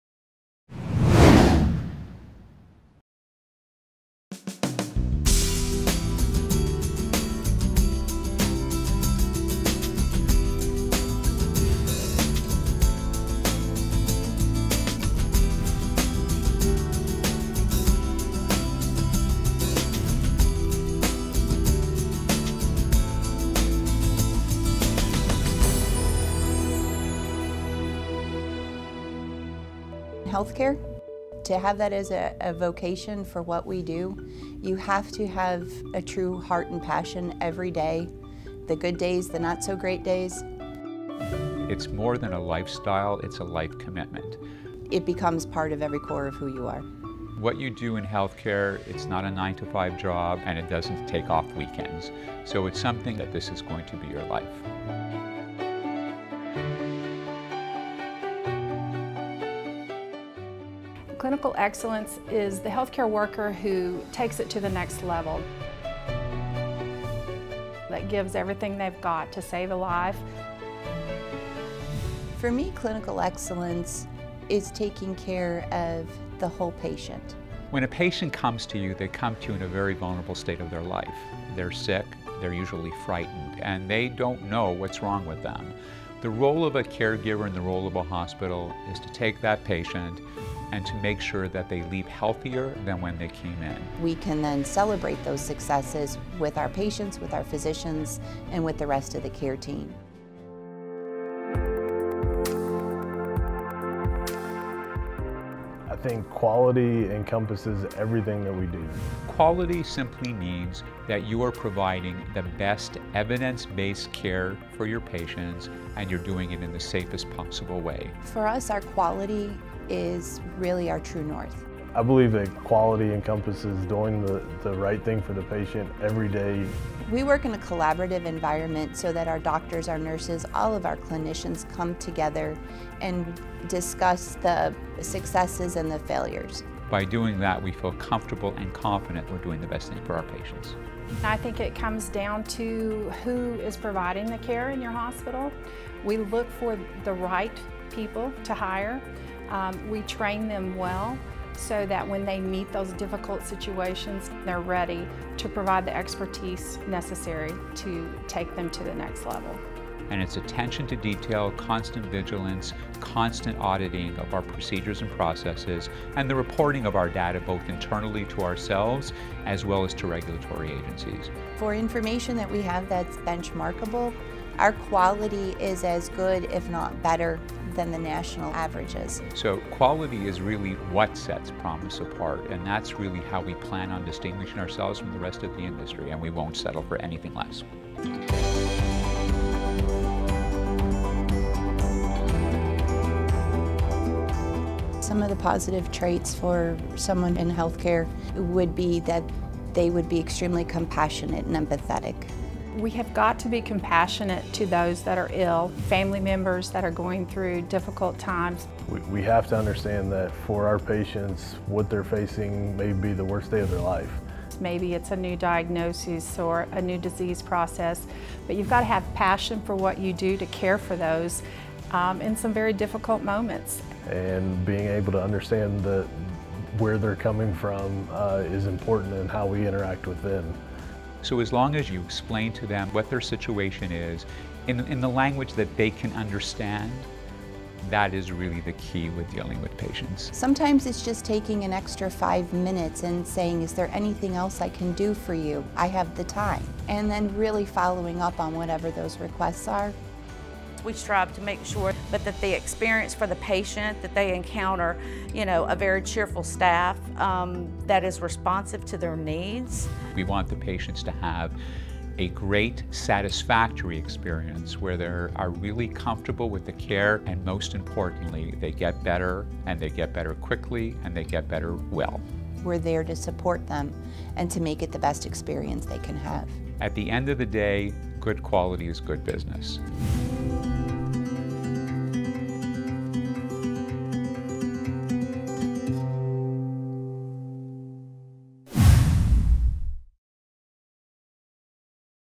Promise Healthcare - Clinically Speaking
Promise Healthcare is a specialty post-acute care health system with 14 hospitals and two skilled nursing facilities across the U.S. Serviced by more than 3,000 physicians and employees, Promise Healthcare facilities are known for outstanding clinical quality scores that consistently outperform national benchmarks. Patient satisfaction ratings at Promise hospitals are among some of the highest in the industry ranging between 91.06% and 96% since 2012.
This corporate video series was produced to give prospective patients and their families an overview of the company, its values and the quality of care they can expect while at a Promise hospital.